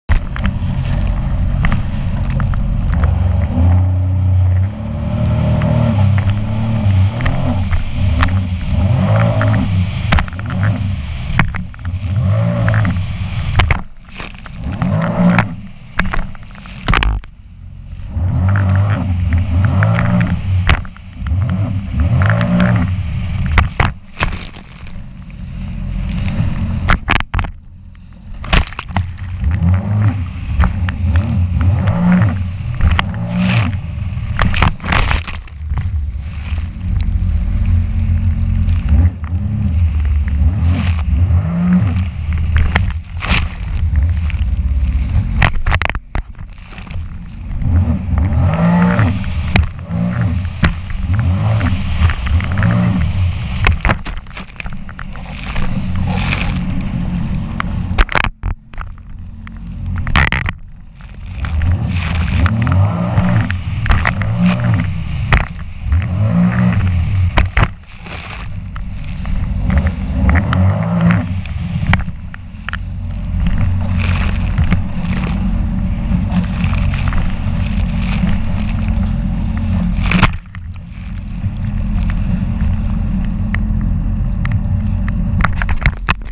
Superman
"Flying" off the coast of Surf City North Carolina